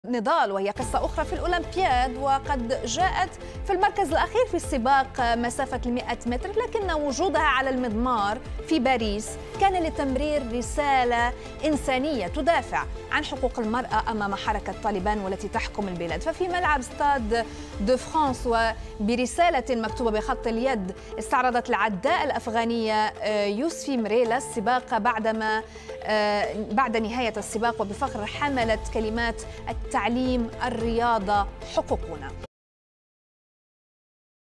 أولمبية أفغانية في رسالة بخط اليد لـ طالبان: "التعليم والرياضة وحقوقنا"
العربية_ويكند | من المركز الأخير في سباق الـ 100 متر... أولمبية أفغانية في رسالة بخط اليد لـ طالبان: "التعليم والرياضة وحقوقنا"
لمعرفة أحدث المستجدات.. عبر البث المباشر من هنا 👇
وبإمكانكم متابعة العربية على منصات التواصل